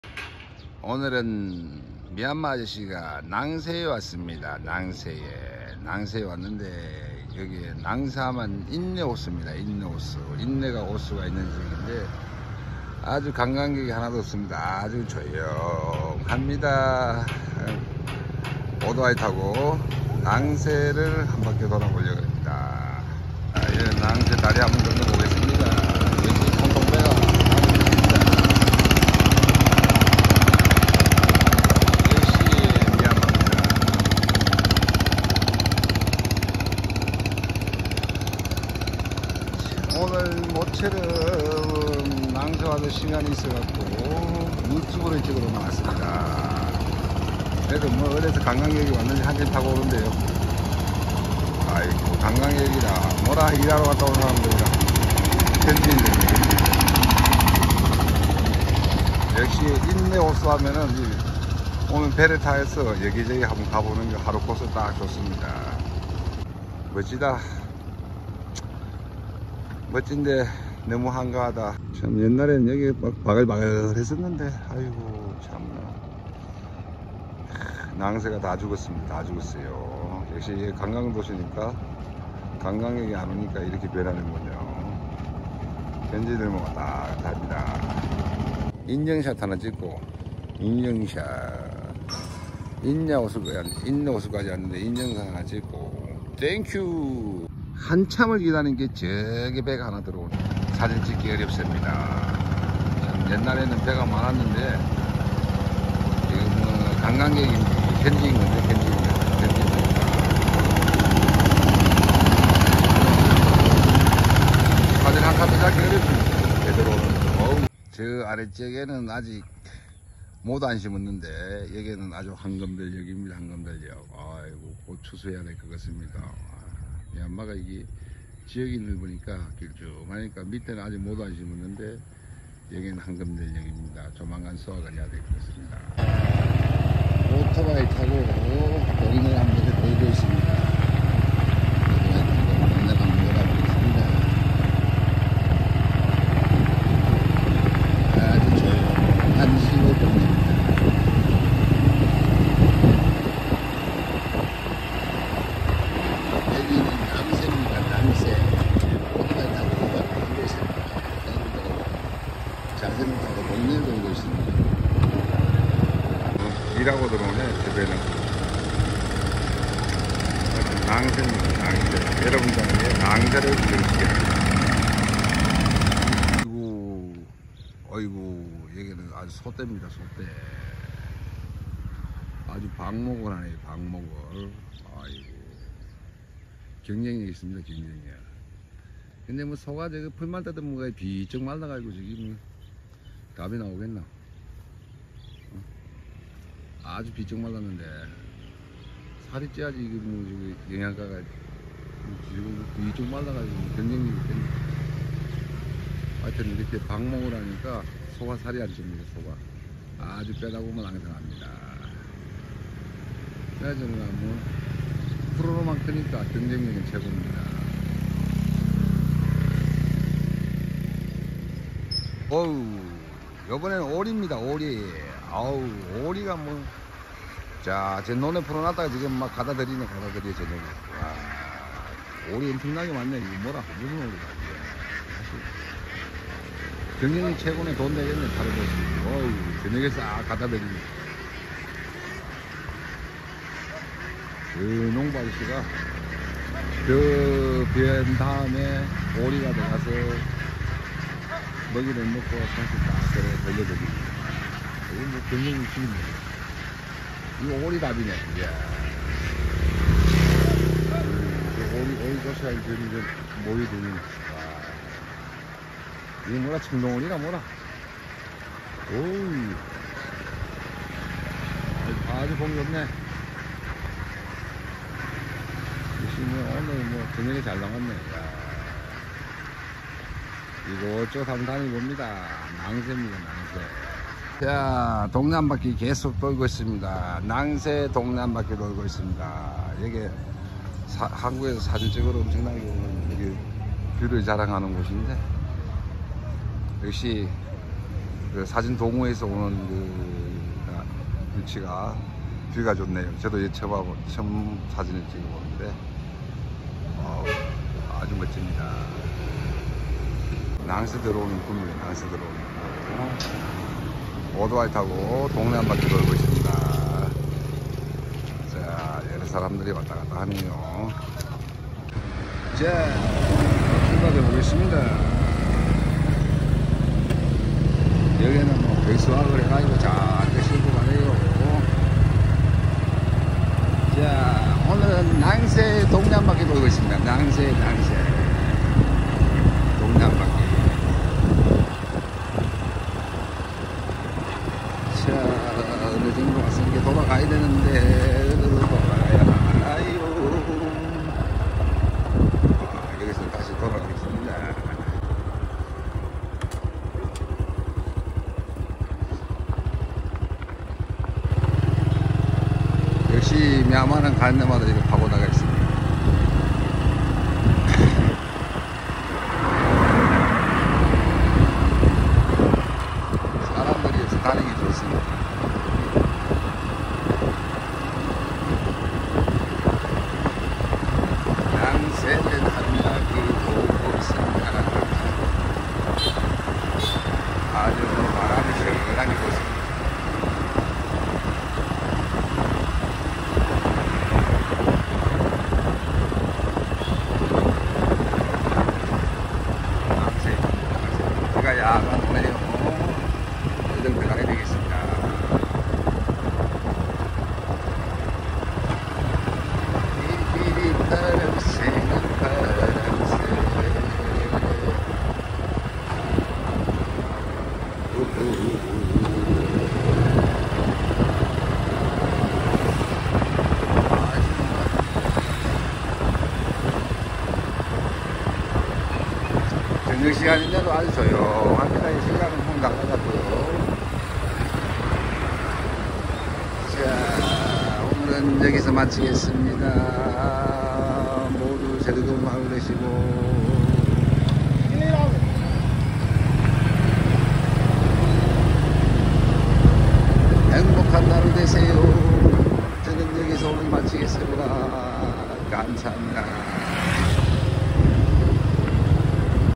미안마 여행지 타웅지 낭쉐 인레호수 동네 한바퀴 ,미얀마 여행 필수코스
미얀마 여행필수 코스, 타웅지 낭쉐 인레호수, 동네 한바퀴 돌아 보았습니다.